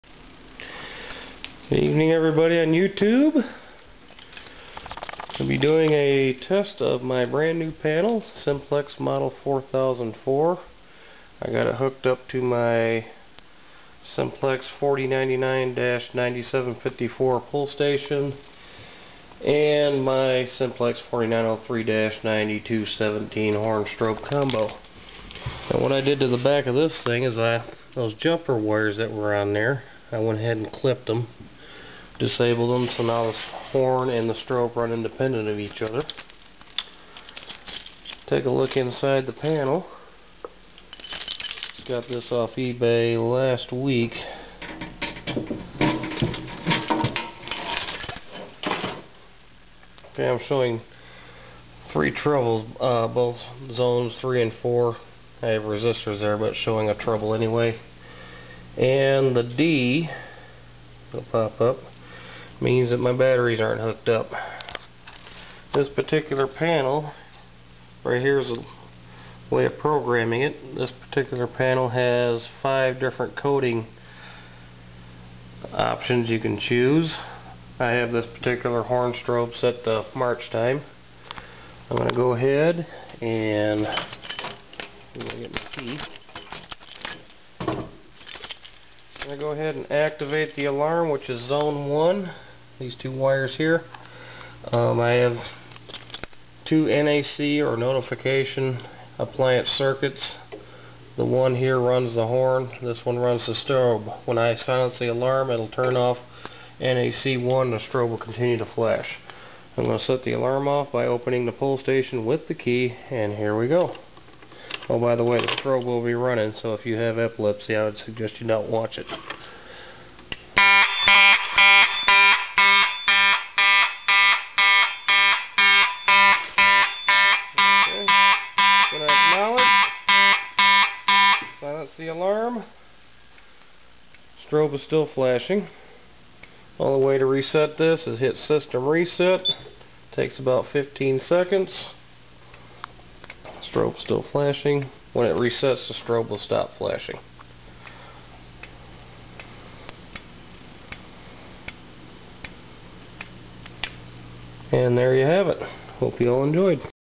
Simplex 4004 Panel Test (March Time)
Test of the Simplex 4004 Fire Alarm Panel.  This is the panel that will run my home system.  It will have 2 zones (even though it's capable of running 4).  Zone 1 will be my three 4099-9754 pulls and Zone 2 will be my smoke detectors.  The horn/strobes will be my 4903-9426 TrueAlerts.  This test is with my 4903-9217 horn/strobe combo just to demonstrate how it works.